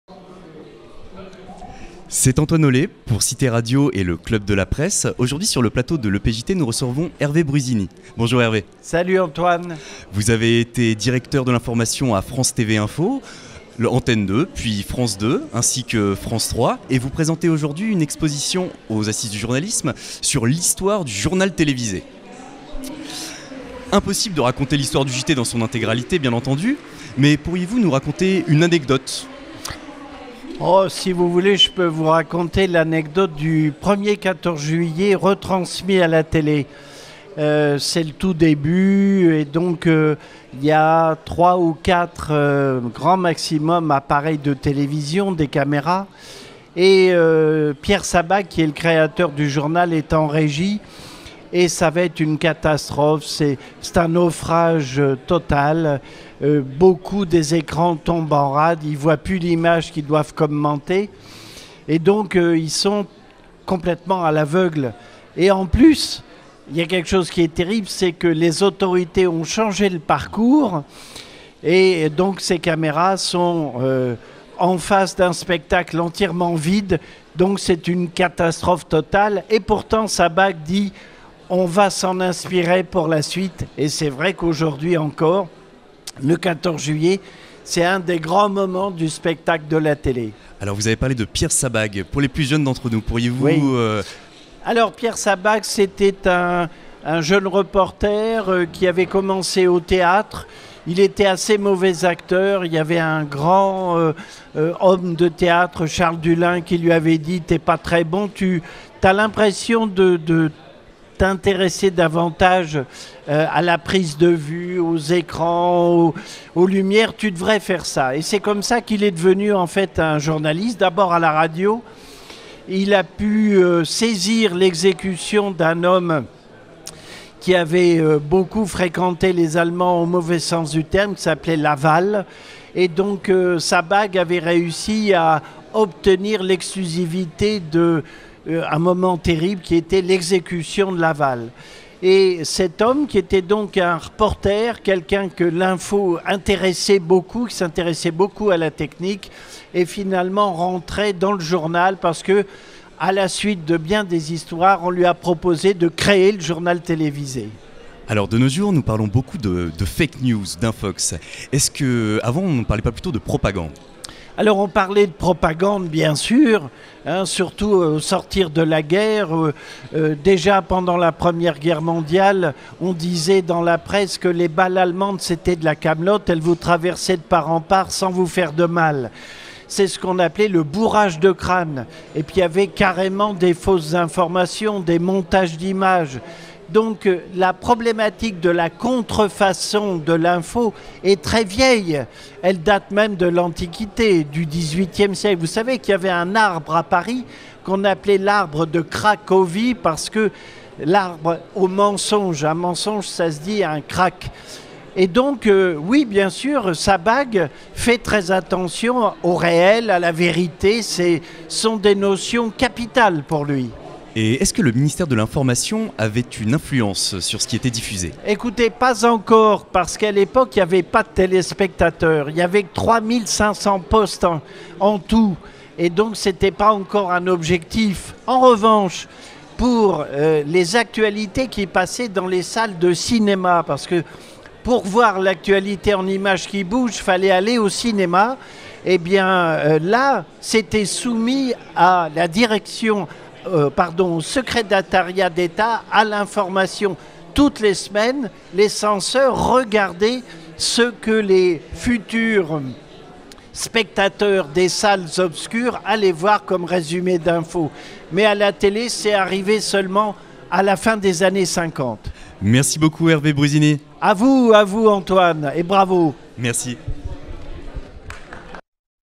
Hervé Brusini raconte... le début de l'info à la télé en 1949
Ancien directeur de l'information de France Télévisions, Hervé Brusini présente aux Assises du Journalisme une exposition sur les 70 ans du 1er JT télé. Une plongée dans l'histoire "artisanale" des débuts de la télé. Où l'on se souvient que les fake news s'appelaient autrefois la propagande. Interview d'Antoine Ollé, service civique pour Citéradio.